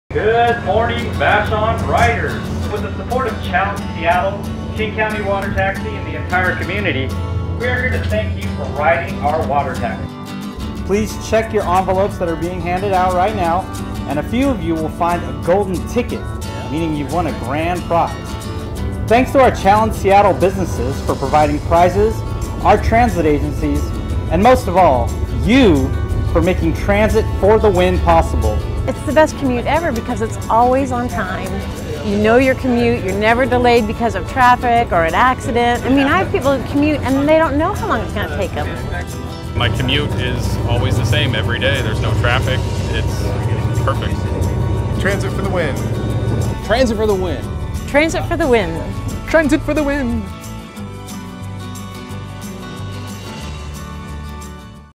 2017 Seattle Commute Giveaway - WATER TAXI
On Wednesday, June 21, 2017, King County Water Taxi and Commute Seattle surprised commuters from Vashon Island with prizes from local businesses. It was part of an effort called Transit For the Win and Ride Transit Month, which celebrates transit users throughout Seattle.